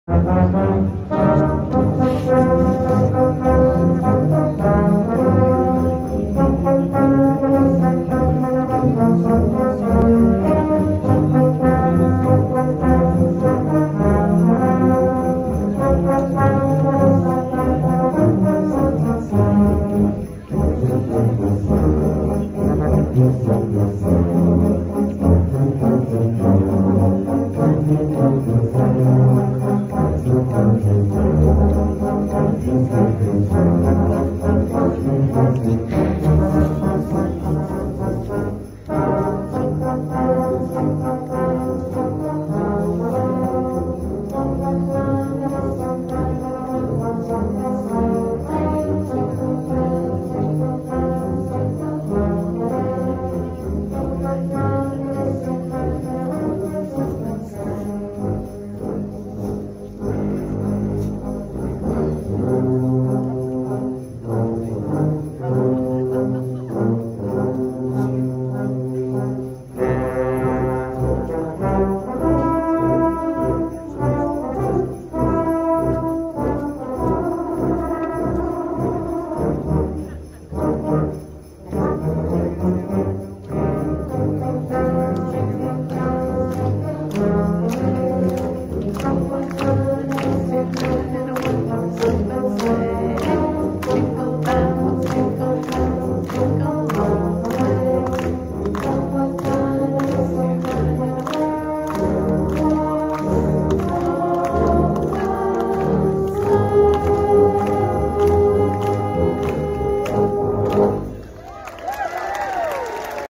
TubaChristmas at UNT Dec. 18, 2021
130 tuba and euphonium players strong.